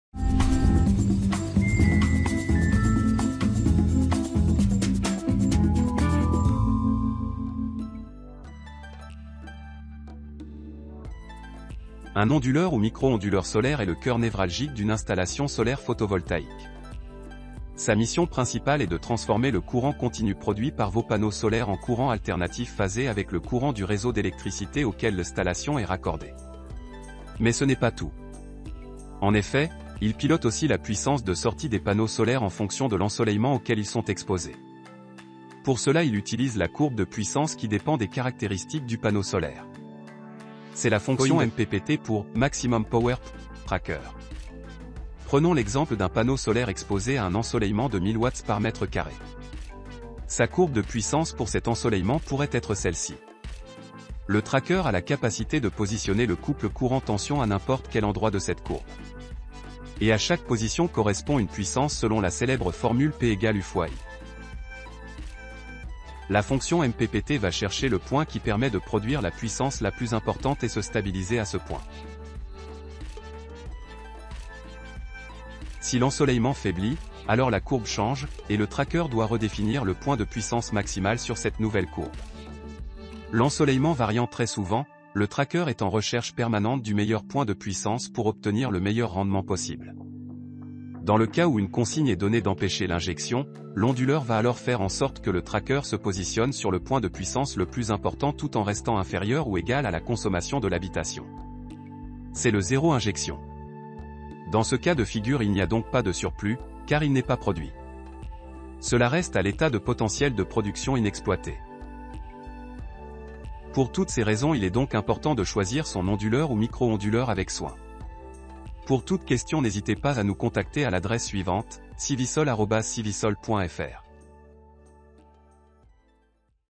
Onduleurs et micro-onduleurs solaires : la fonction MPPT et le 0 injection
Civisol vous présente la fonction MPPT des onduleurs et micro-onduleurs solaires.

Pièce centrale de votre installation solaire, l’onduleur ne fait pas que transformer le courant continu produit par vos panneaux solaires en courant alternatif mais grâce à la fonction MPPT il pilote la puissance de sortie des panneaux solaires en fonction de l’ensoleillement.

Il peux même vous permettre d’empêcher l’injection de votre surplus sur le réseaux en faisant en sorte que vos panneaux ne produisent pas plus que ce que vous consommez.